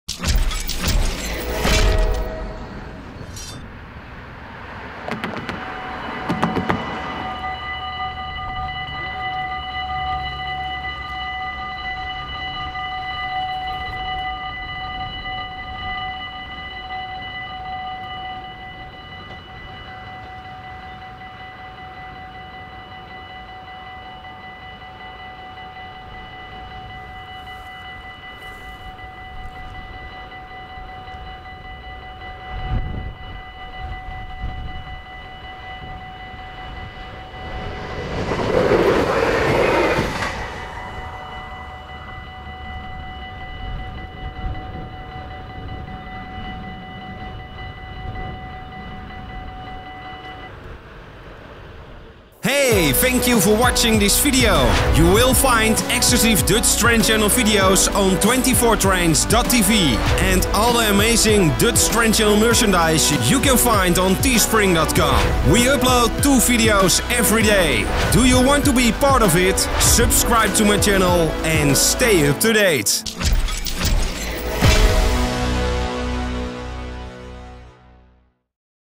DUTCH RAILROAD CROSSING - Sint Odiliënberg - Groot Bergerweg
A dutch railroad crossing at Sint Odiliënberg  - Groot Bergerweg

We zetten ook regelmatig (uniek) beeldmateriaal op Shutterstock dat u zonder tussenkomst van FP Video kunt kopen en downloaden